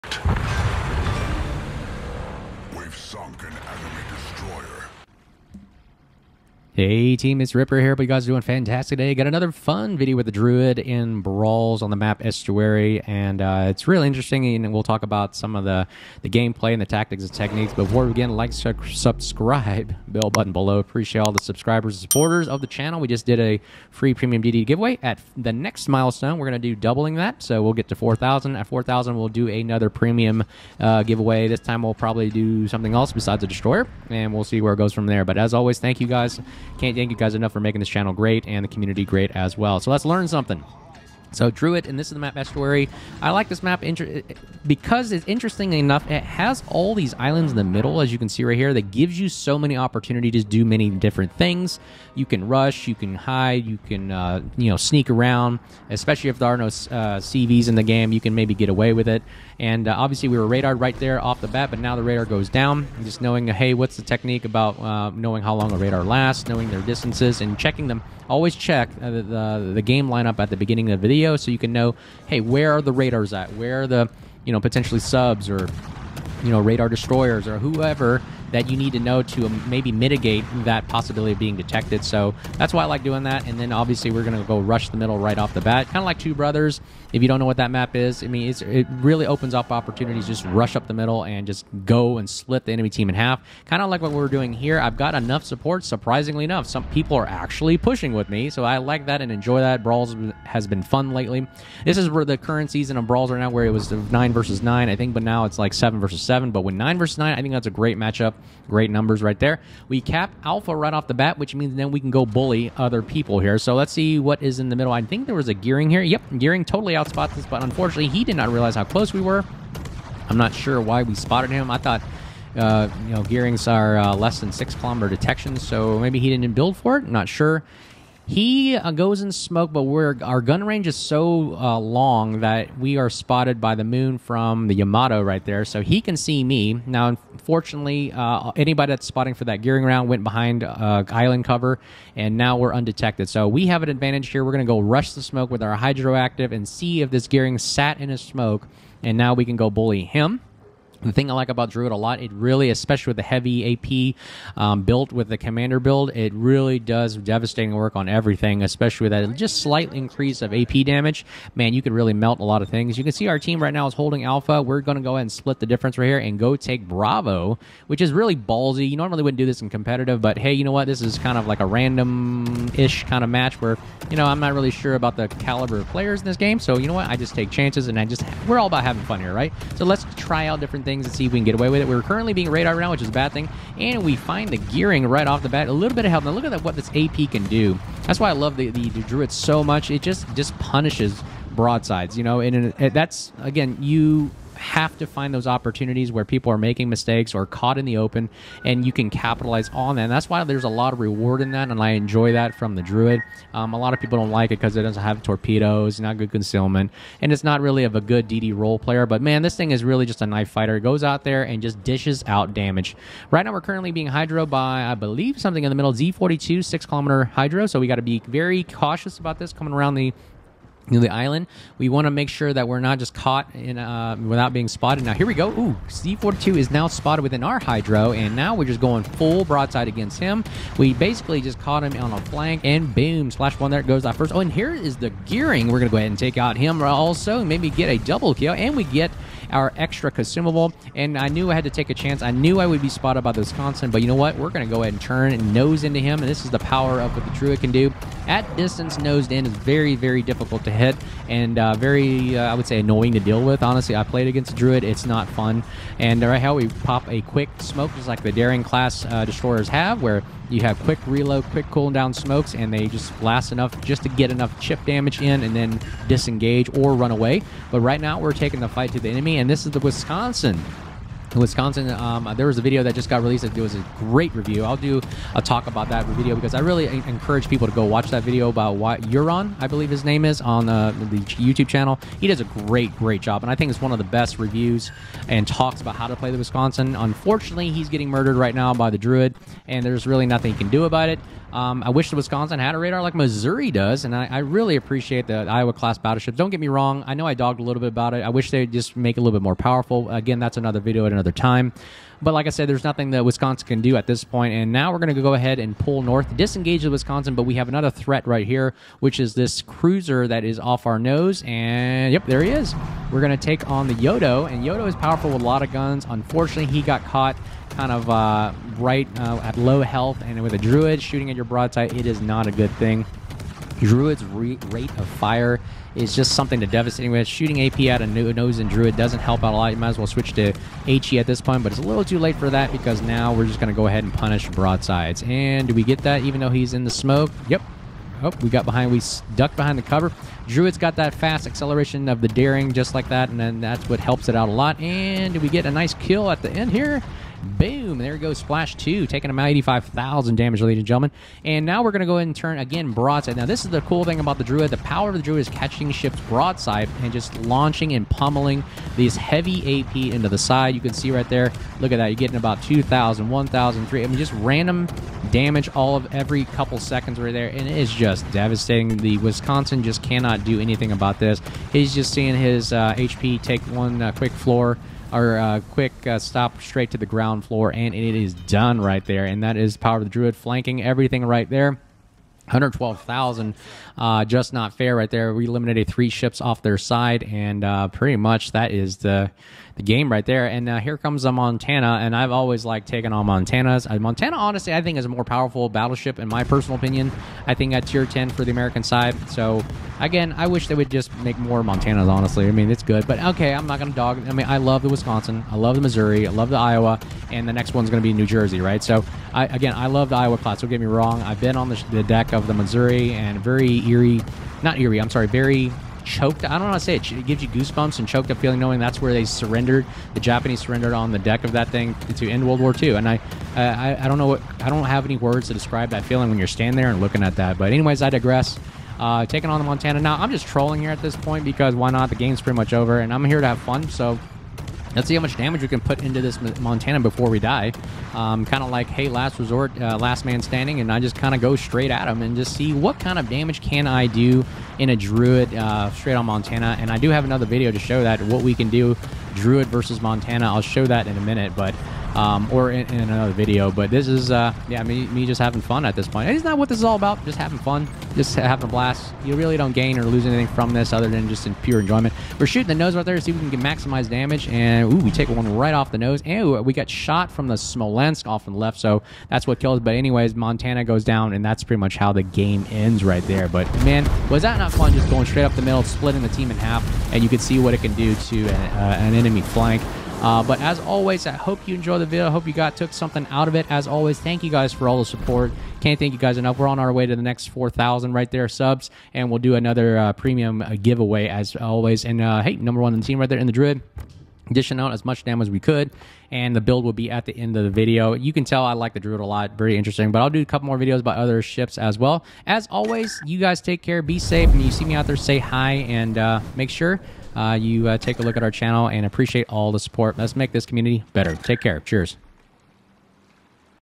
Destroyer Druid up the middle, flanking everyone, nothing but net
Enjoy the gameplay and observe the tactics.